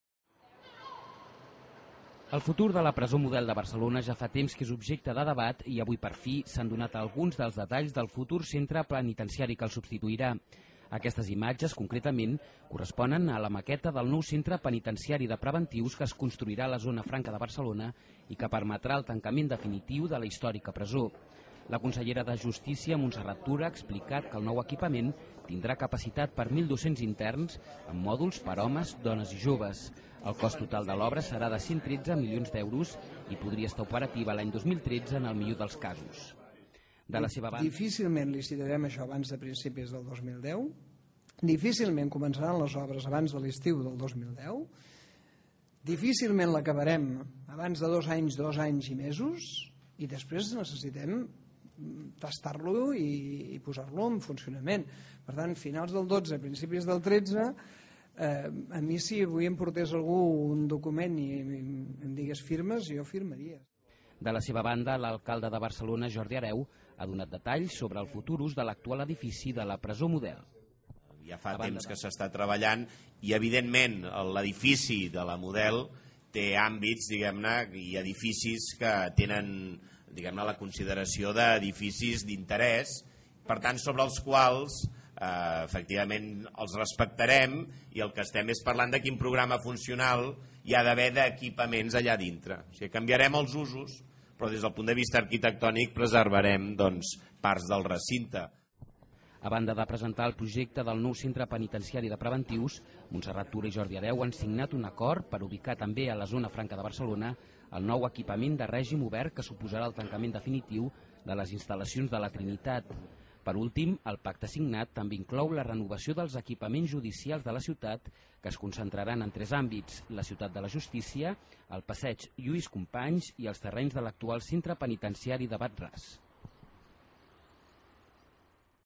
La presó de la Zona Franca tindrà capacitat per a 1.200 interns i costarà uns 115 milions d'euros
Barcelona (ACN).- La presó de preventius situat a la Zona Franca de Barcelona tindrà una capacitat per a 1.200 interns i costarà uns 115 milions d'euros entre l'enderroc dels actuals edificis i la construcció del nou centre. El projecte preveu 677 cel·les, la majoria de dues persones. Aquest equipament ha de substituir l'actual presó coneguda com la Model, i trasllada un centre penitenciari fora del teixit urbà de Barcelona. Tot i això, la ciutat tindrà dos centres oberts de compliment de condemna. Un estarà situat també a la Zona Franca molt a prop de la futura presó, mentre que l'altre es construirà al costat de la futura seu del Departament de Justícia on actualment s'ubica la presó de dones de Wad Ras.